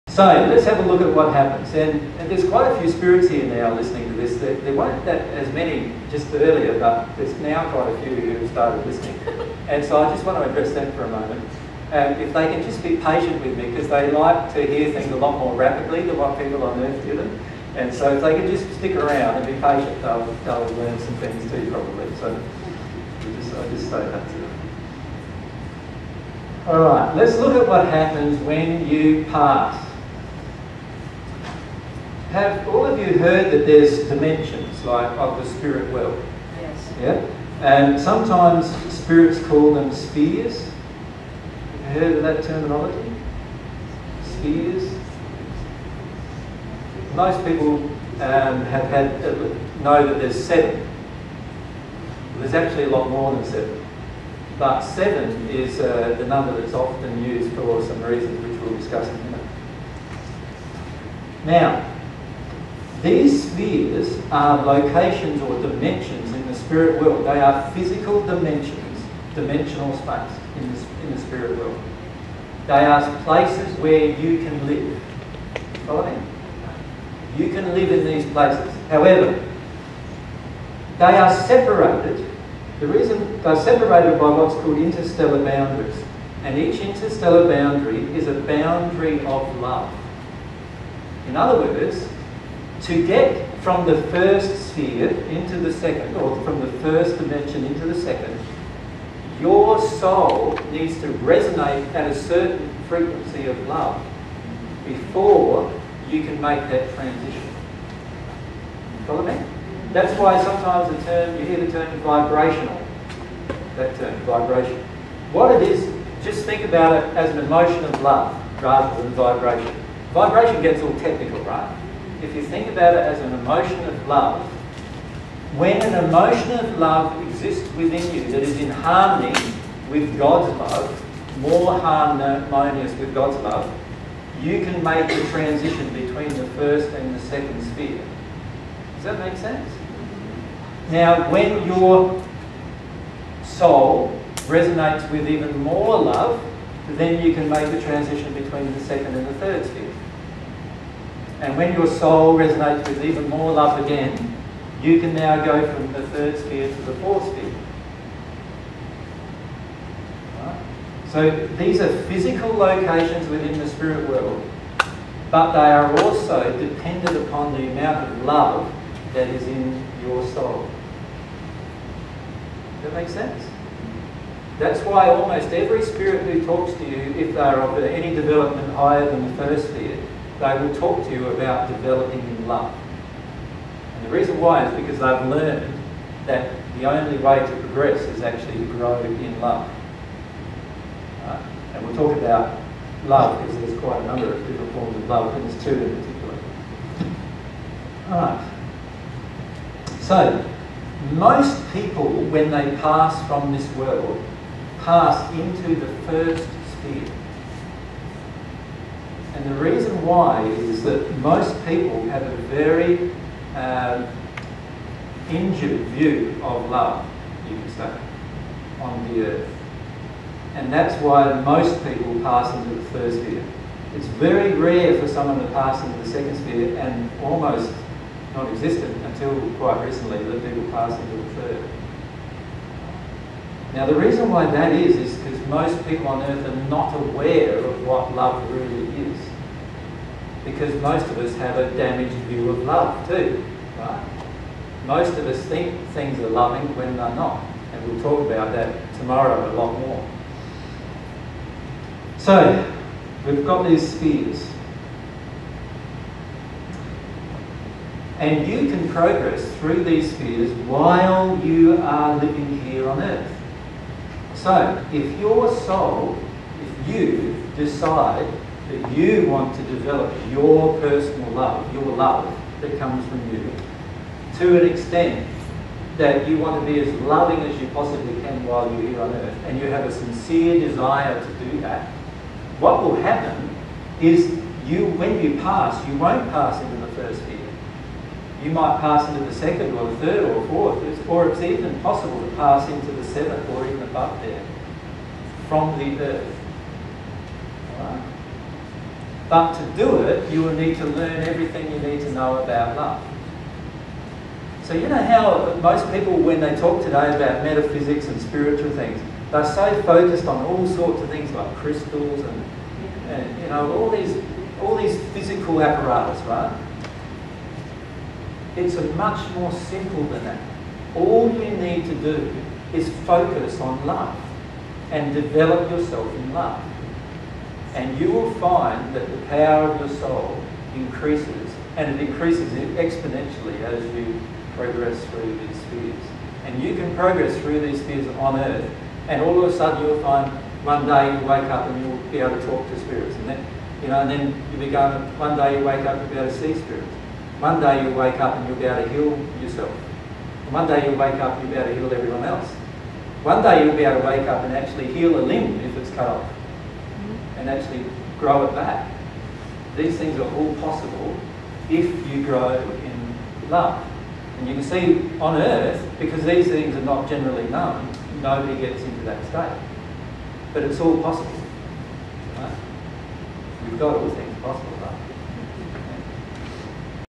Jesus on Traversing the Spheres of the Spirit World
Jesus discusses what happens when we pass and how progression through the spheres of the spirit world is based on the condition of love in the soul.

This is a section of the presentation by Jesus entitled "20070914 General Discussion - Q&A From People In Cocoa S1P1".

dtstart:004420:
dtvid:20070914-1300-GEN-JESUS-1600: